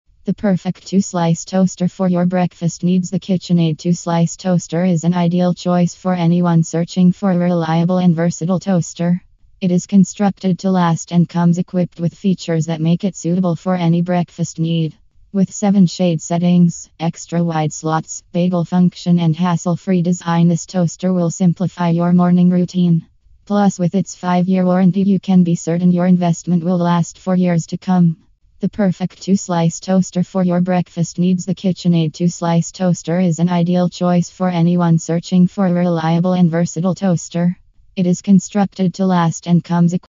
TOP-RATED FOOD PROCESSORS FOR EFFORTLESS COOKING
A food processor is an indispensable kitchen tool for anyone who enjoys cooking. It makes food preparation simpler, faster and more efficient by chopping, blending, pureeing and shredding ingredients in just minutes - giving you delicious meals with minimal effort. Food processors come with a range of features and capabilities, making it difficult to choose the one best suited for your needs. To help you decide, we have compiled this list of KitchenAid's top-rated and the best food processors for effortless cooking